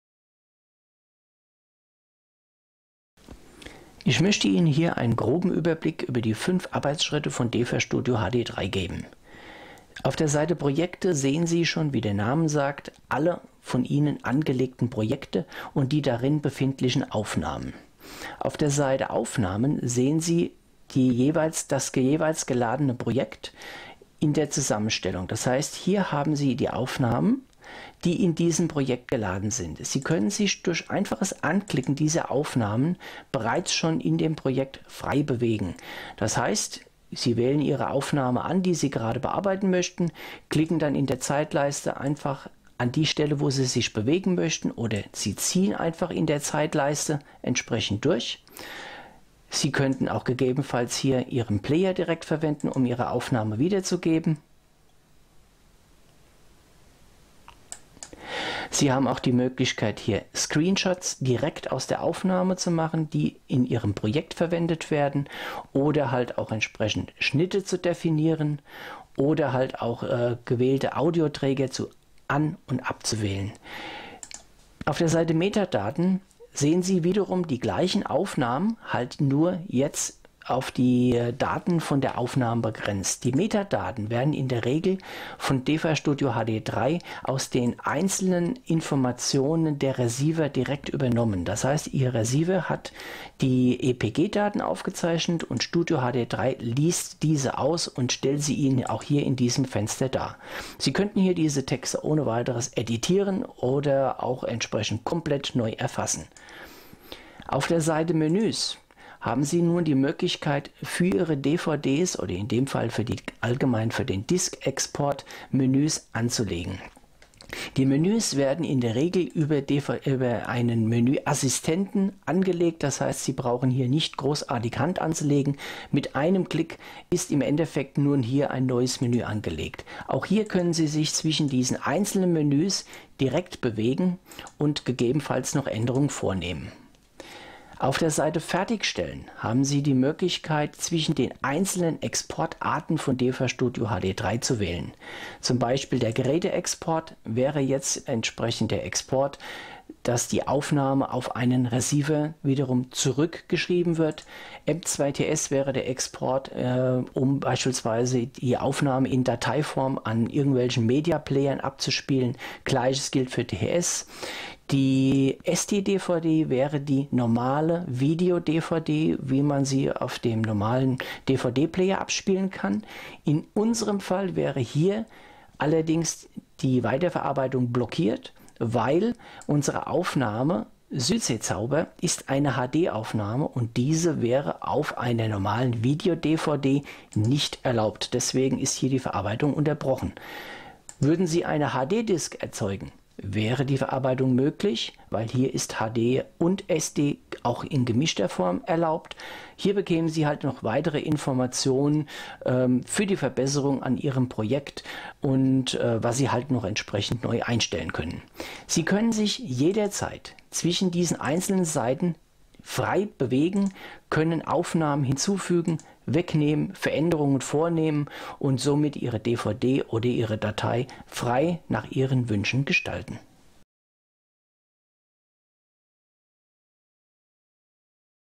HD3: Die 5 Hauptbereiche
HINWEIS: Video zeigt HD3. Identische Oberfläche und gleiches Vorgehen in allen Folgeprodukten.
Beschreibung:
Dieses Video stellt Ihnen die fünf Verarbeitungsschritte in DVR-Studio HD3 vor:
- Projekte: Verwalten der Aufnahmen.
- Aufnahmen: Bearbeitungsfenster der Aufnahmen
- Meta-Daten: Bearbeiten der Filminformationen für die verschiedenen Export-Arten.
- Menü: Automatisierte Erstellung der Menüs beim Disk-Export.
- Fertigstellen: Auswahl der Exportarten mit den Zusatzoptionen der einzelnen Exportart.
Laufzeit : 3:48 Min